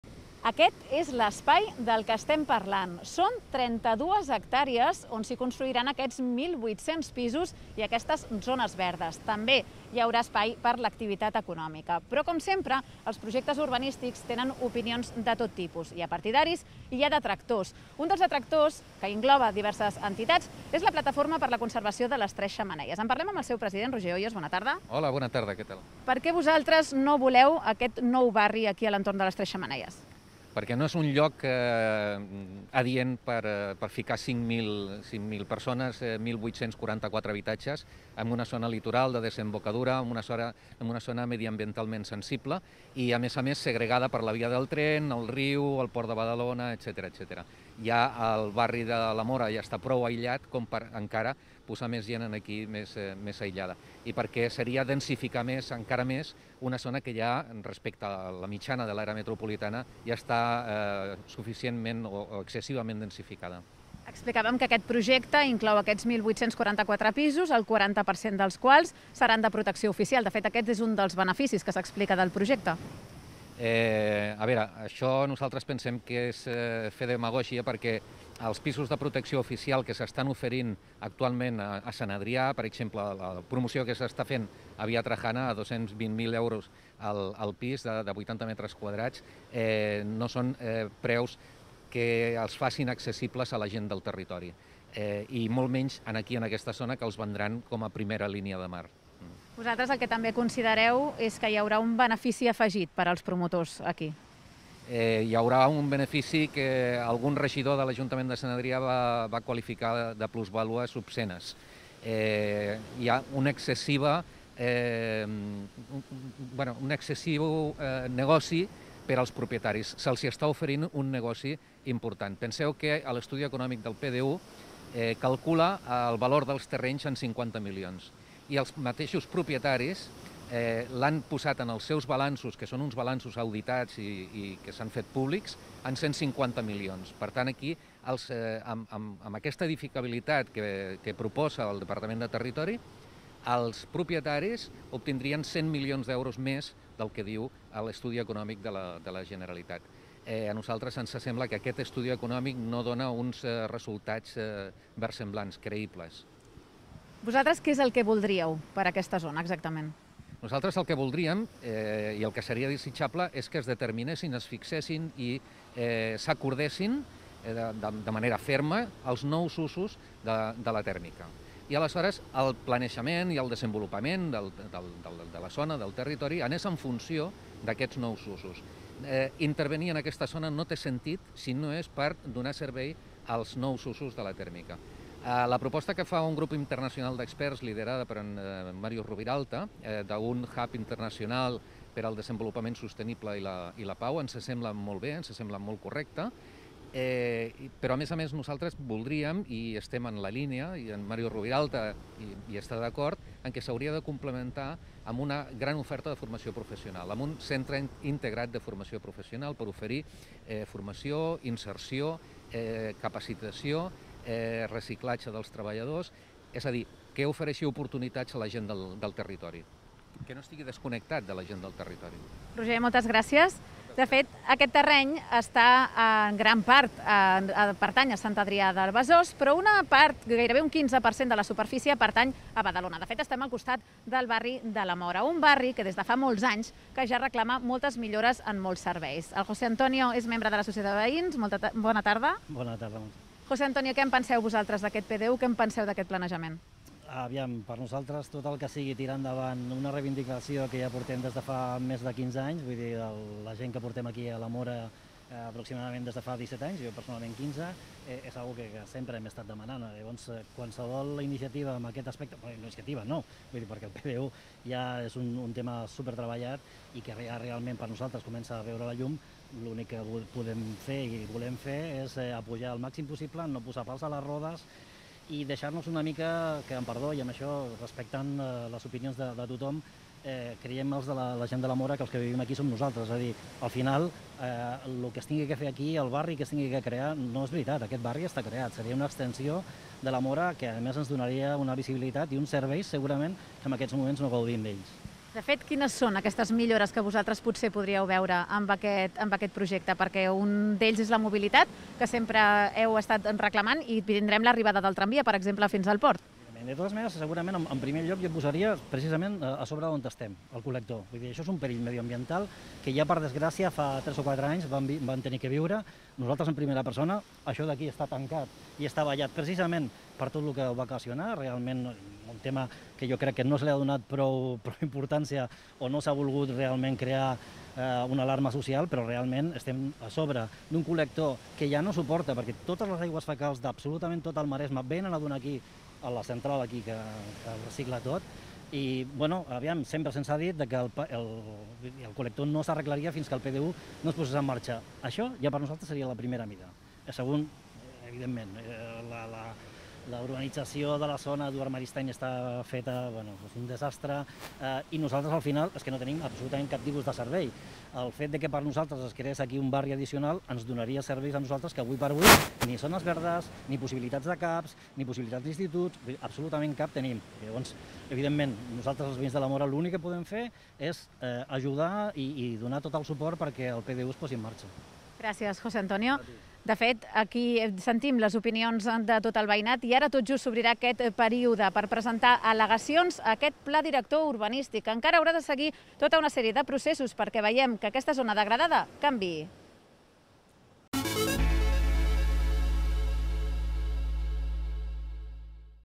S'obre el període d'al·legacions per al projecte ubranística de les 3 xemeneies - Badalona Tres60
Aquesta setmana s'ha obert el període d'al·legacions per al projecte urbanístic que canviarà radicalment l'entorn de les 3 xemeneies de Sant Adrià de Besòs. La Montse López ens ensenya el terreny on es construiran 1.844 pisos i zones verdes i coneixem l'opinió del veïnat d'aquest Pla Director Urbanístic.